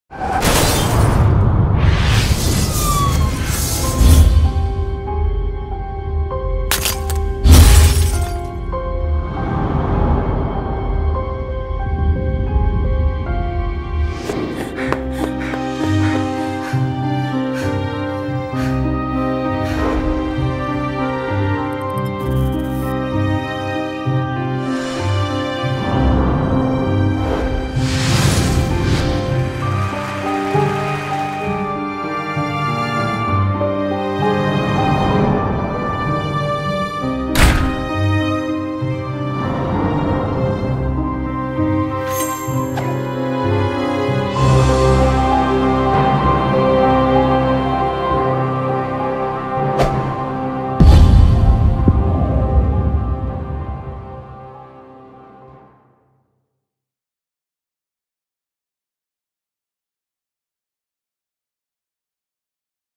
Ghost Theater (Official Trailer) | WEBTOON
The shock of a tragic accident leaves talented actress Rahee Ku unable to make facial expressions. Her acting career seems hopeless... until she discovers the mysterious Ghost Theater.